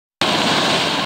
P1011162
Short video of FF clearing the bird after loading a patient from bad MVA in Tewksbury MA